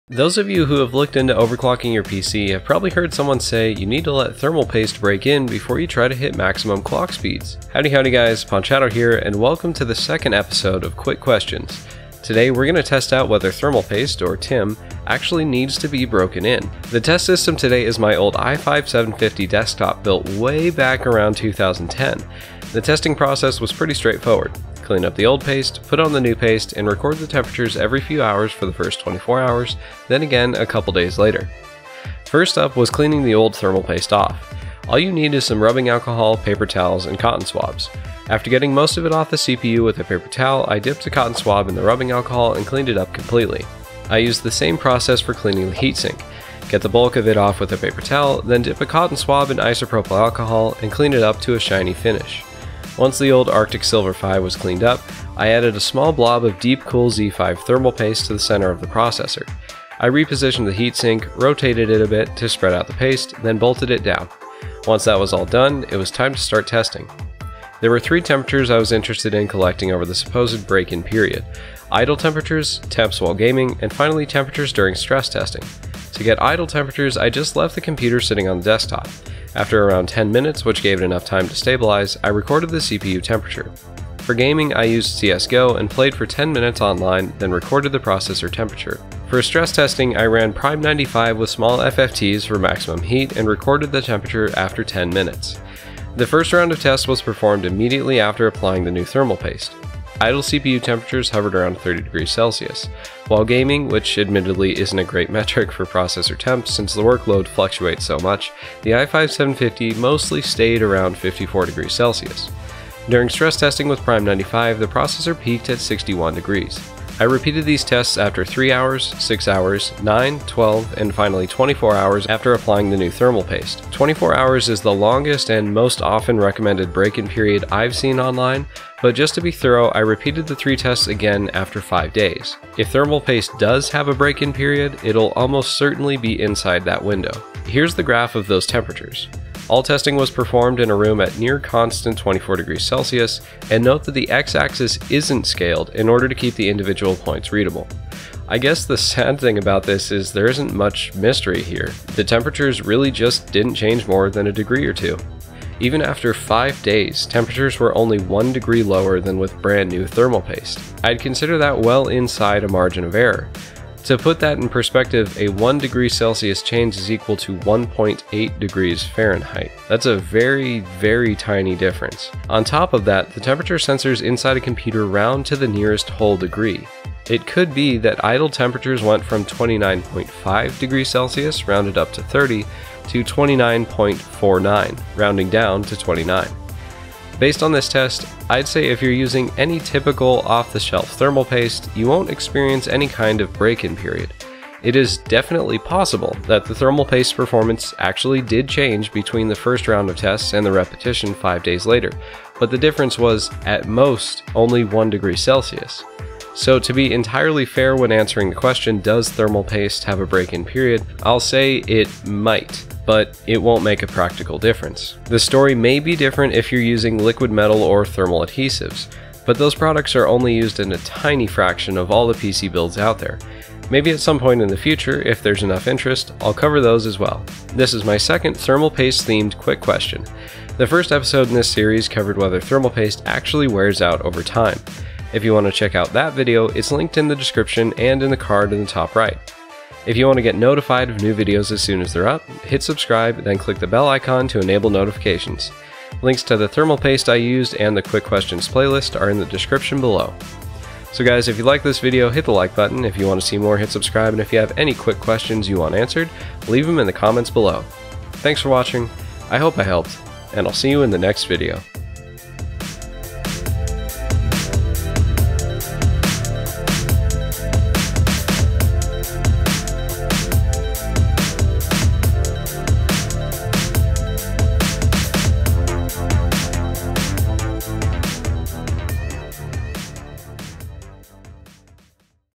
Quick Question 002: Does Thermal Paste Need To Be Broken In?
The long-lauded “break in period” for thermal paste… Is it real? Does it even make a difference? Find out in this video!

Deepcool Z5 Thermal Paste on Amazon
Deepcool GAMMAXX 400 on Amazon